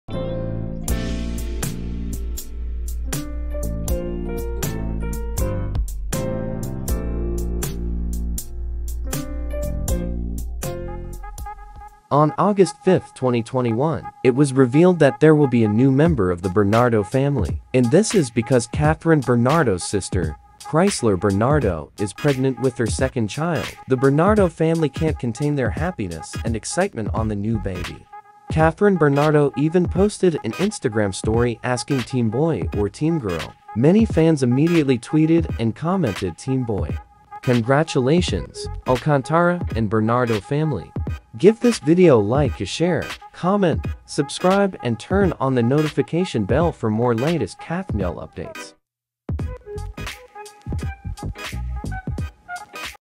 UPDATE 08/05: NEW BERNARDO FAMILY MEMBER! Who’s pregnant? 🤍
On August 5, 2021, It was revealed that there will be a new member of the Bernardo family and this is because Kathryn Bernardo’s sister, Chrysler Bernardo is pregnant with her second child. The Bernardo family can’t contain their happiness and excitement on the new baby. Kathryn Bernardo even posted an Instagram story asking with a caption “Team Boy or Team Girl?”

What do you think? Will it be a girl or boy? 💗💙
💿 Follow KathNiel 💿

🎞 ᗰOᖇᗴ 🎞